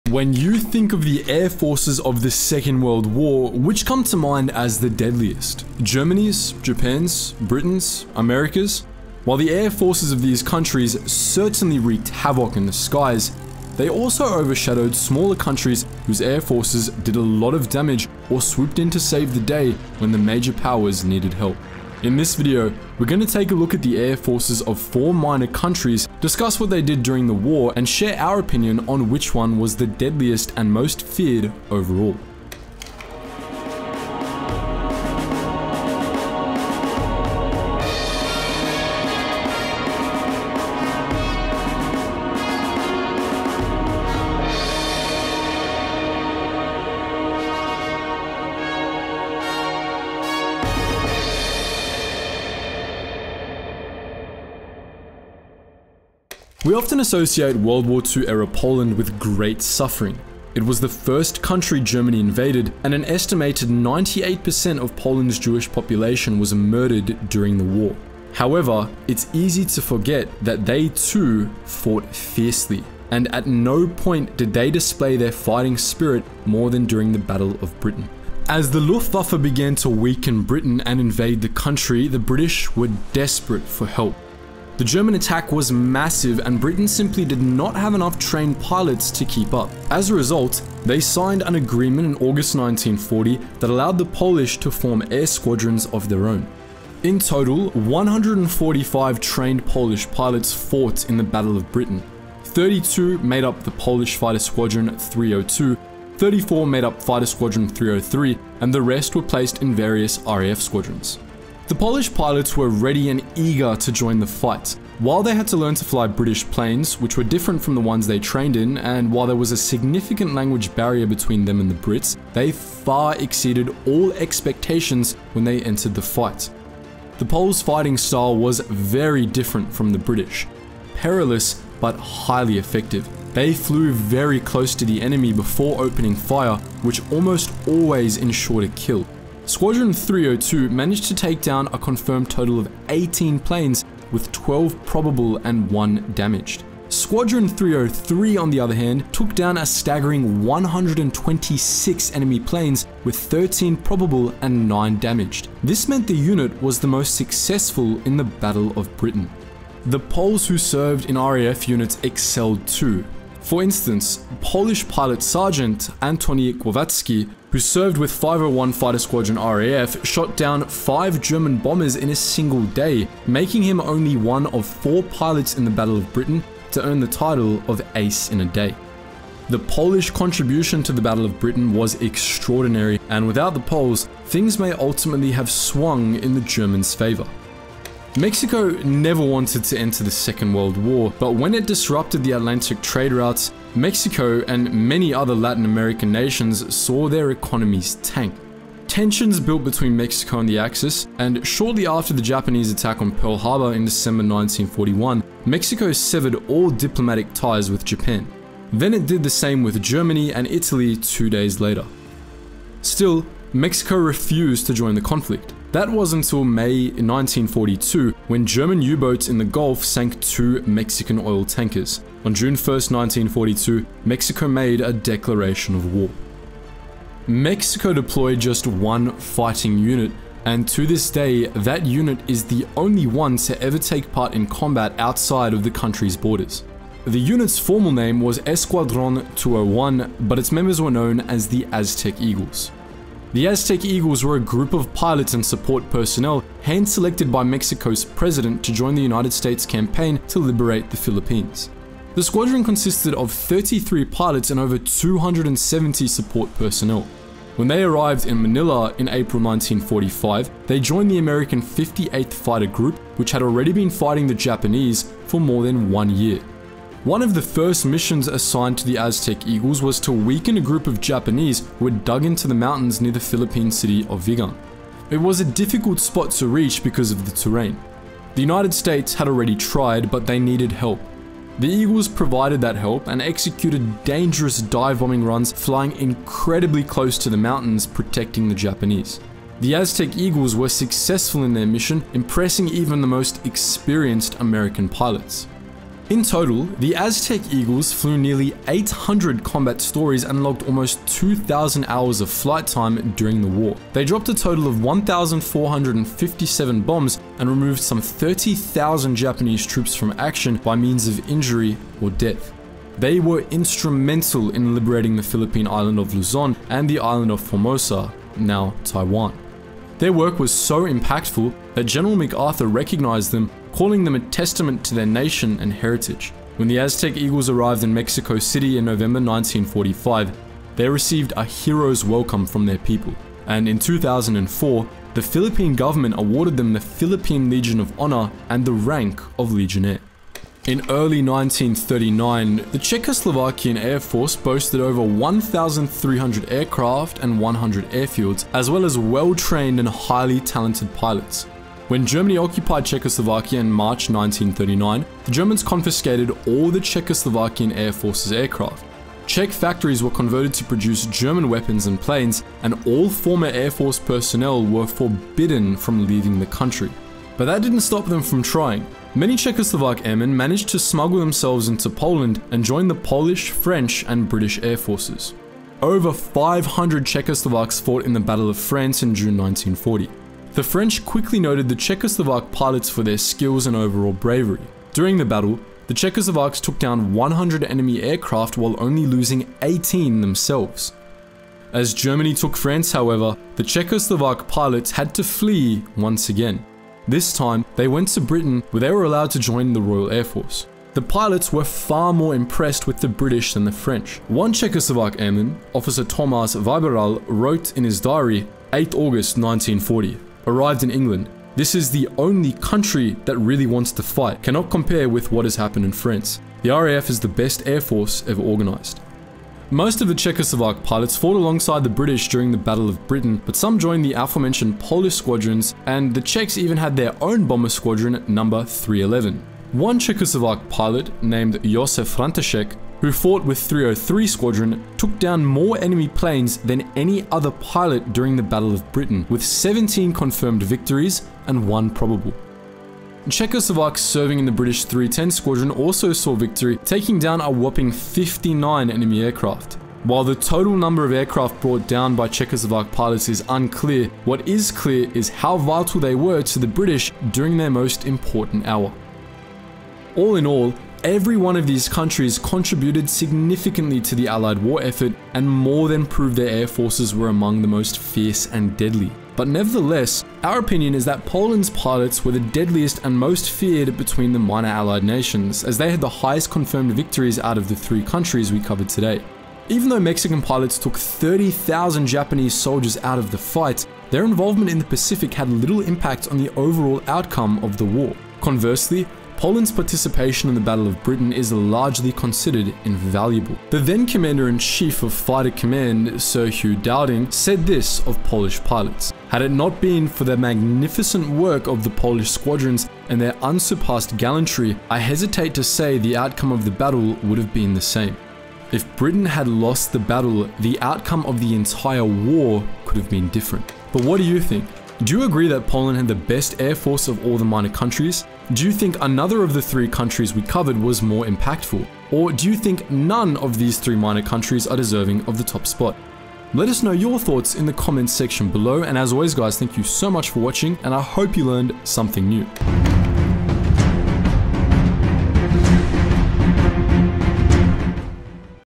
Which Minor Country had the DEADLIEST & Most FEARED Airforce
While the German and British Air Forces had plenty of modern planes and skilled pilots to make themselves deadly during the Second World War, they were not the only countries who had feared air forces. A number of minor countries whose air forces did a lot for the war effort. In this video, we talk about three of those countries and what they did to instil fear in the hearts of their enemies.

🎬Video Credits:
Narrator - Cam
Editors - Kshitiz, Shantanu koli
Writer - Isabella
Researcher - Daniel

Chapters

0:00 Introduction
0:55 Poland
3:07 Mexico
5:40 Czechoslovakia
7:39 Conclusion